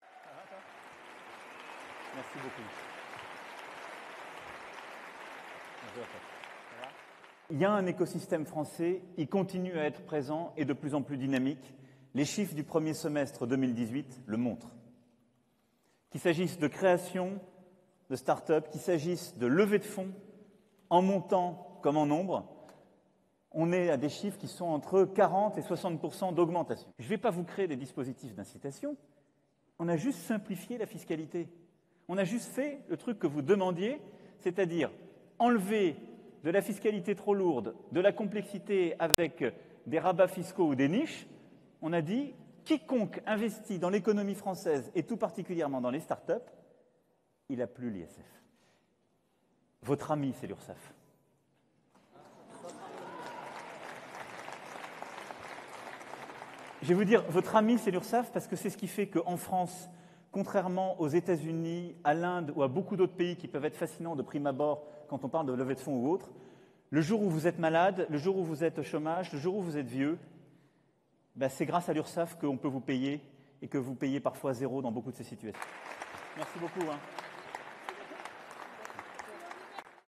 Macron aux start-up: l'Urssaf est "votre amie"
L'Urssaf est "votre amie", assure Emmanuel Macron mardi 09 octobre devant un parterre de dirigeants de start-up lors d'une rencontre à la Station F, pépinière géante de start-up, en vantant le rôle de la Sécurité sociale "quand on est malade, au chômage ou vieux". Durée: 01:20